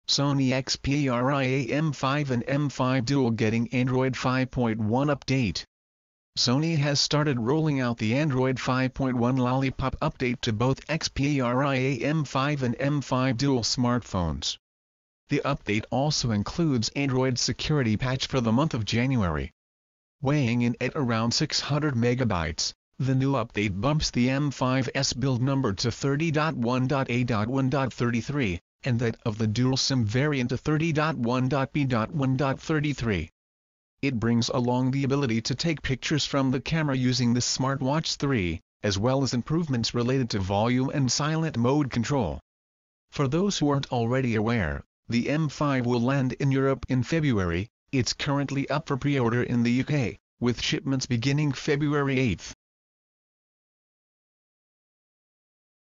Sony Xperia M5 and M5 Dual getting Android 5.1 update
Sony has started rolling out the Android 5.1 Lollipop update to both Xperia M5 and M5 Dual smartphones. The update also includes Android security patch for the month of January.

Weighing in at around 600MB, the new update bumps the M5's build number to 30.1.A.1.33, and that of the dual-sim variant to 30.1.B.1.33. It brings along the ability to take pictures from the camera using the SmartWatch 3, as well as improvements related to volume and silent mode control.

For those who aren't already aware, the M5 will land in Europe in February - it's currently up for pre-order in the UK, with shipments beginning February 8.